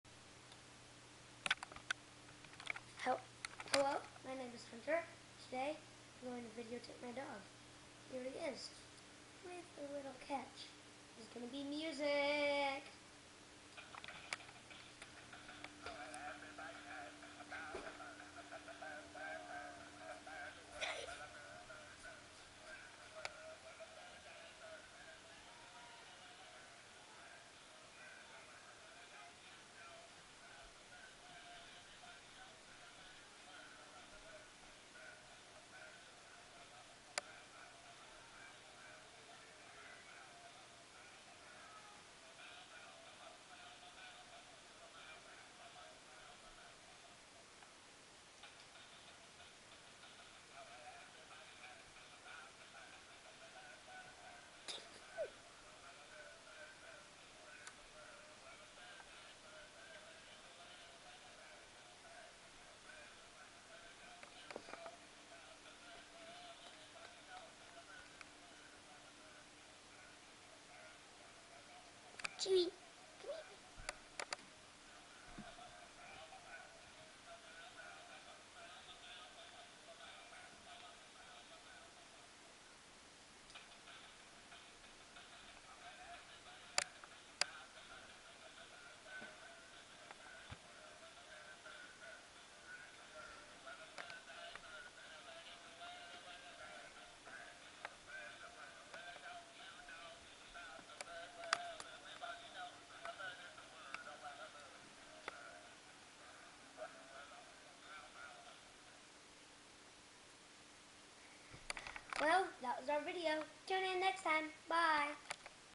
fancy pitbull
Man videos dog that hides in corner, and other funny stuff. fancy music included.